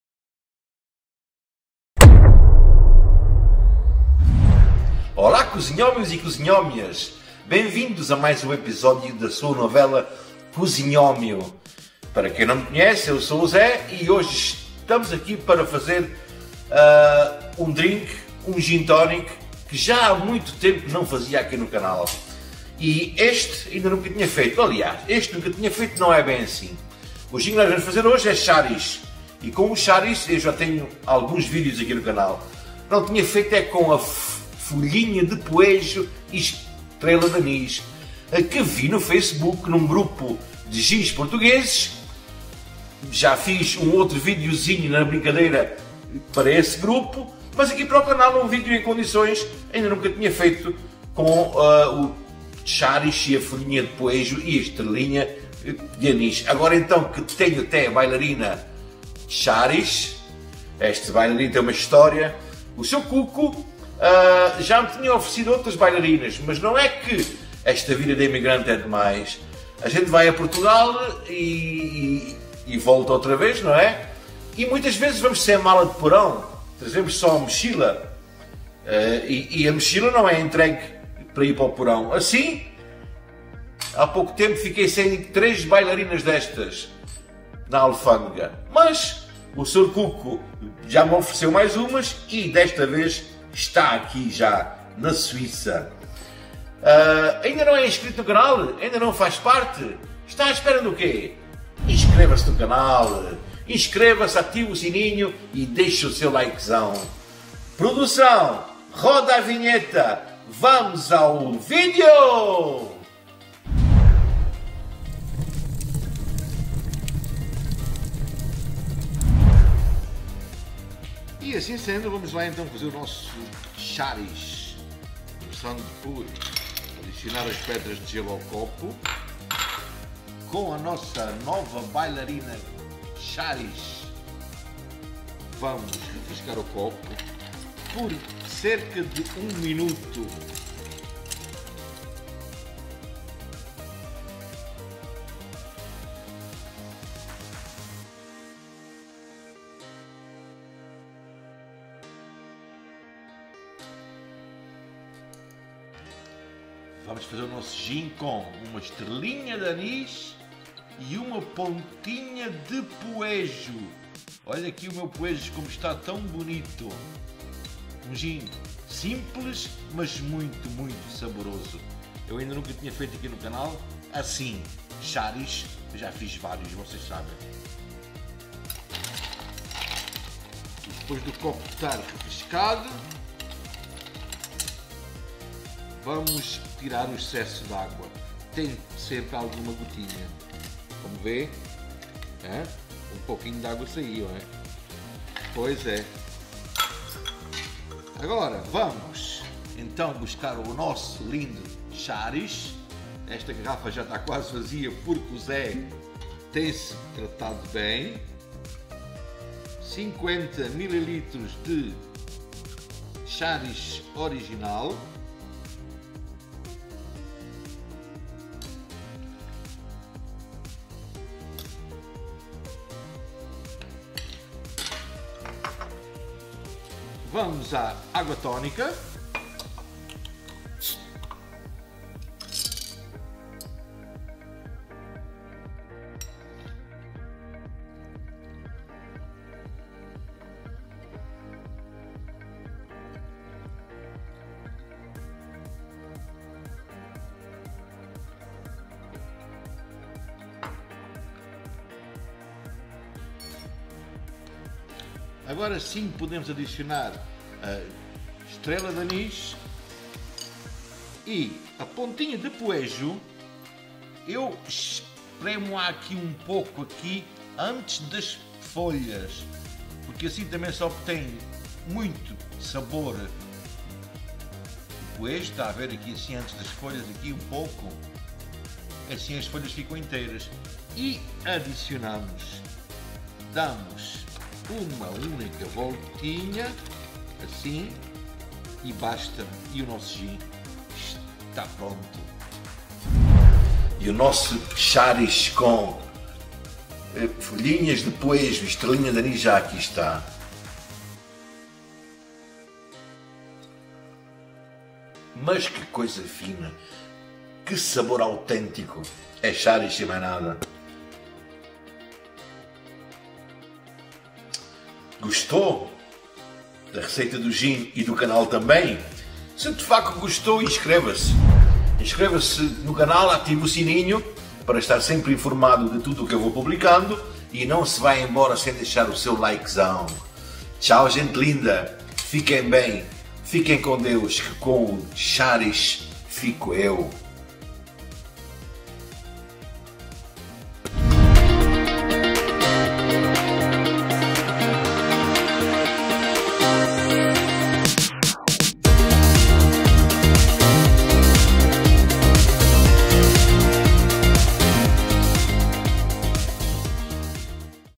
POEJO & ESTRELA DE ANIS | SHARISH GIN | Lentamente destilado no Alentejo
INGREDIENTES:
- 50 Ml. de Gin Sharish
- 200 Ml. de Água tonica
- 1 Estrela de anis
- Raminho de Poejo
- Gêlo q.b.

TXIN TXIN IoI